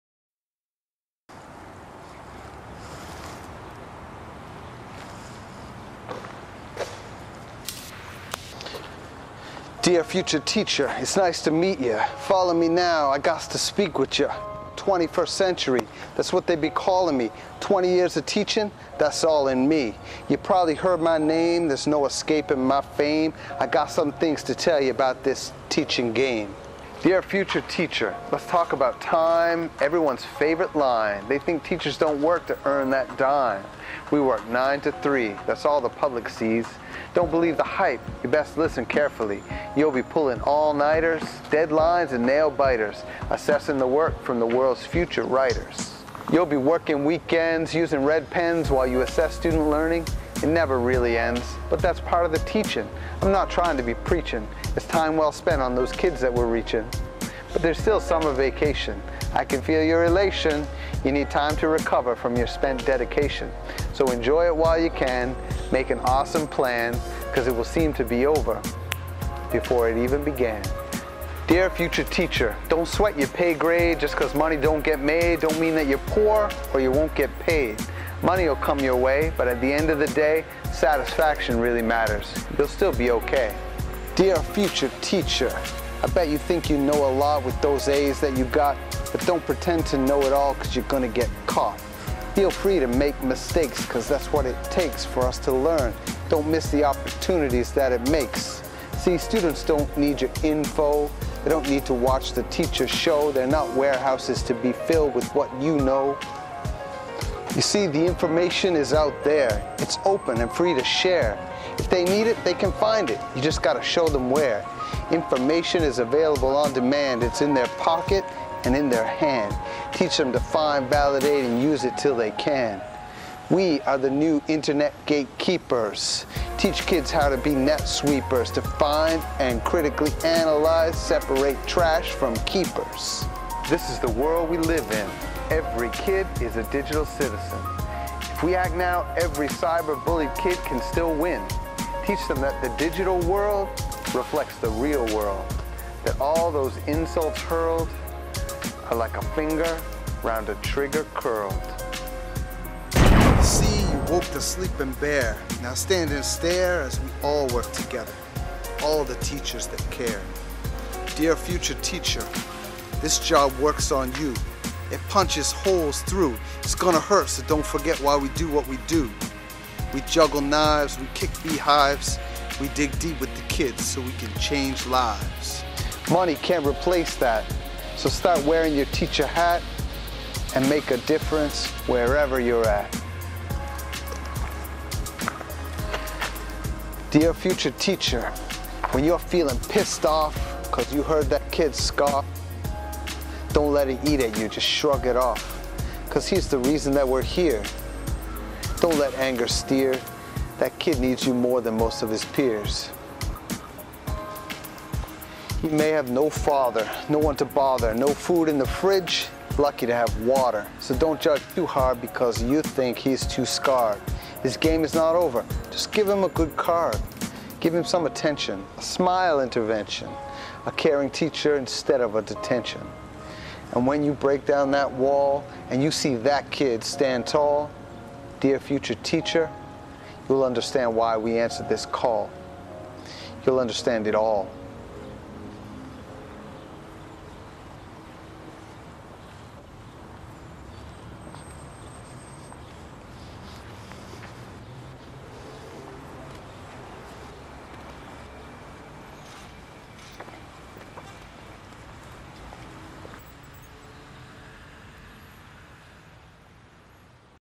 Dear Future Teacher
A spoken word poem of advice for Future Teachers. Written by Troy Messenger. Music by DJ Rostej